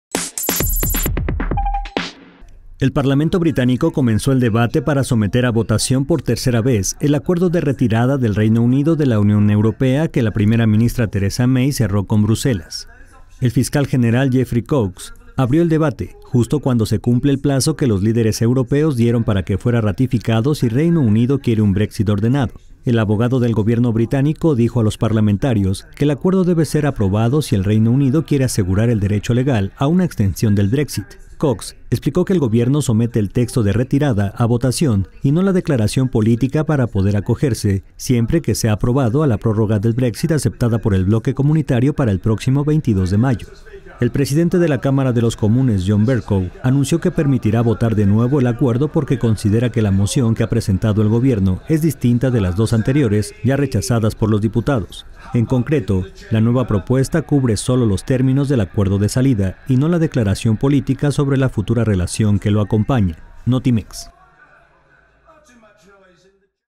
Parlamento británico inicia debate para votar el Brexit
El Parlamento británico comenzó la mañana de este 29 de marzo  el debate para, posteriormente, someter a votación, por tercera vez, el acuerdo de retirada del Reino Unido de la Unión Europea (UE), que la primera ministra, Theresa May, cerró con Bruselas.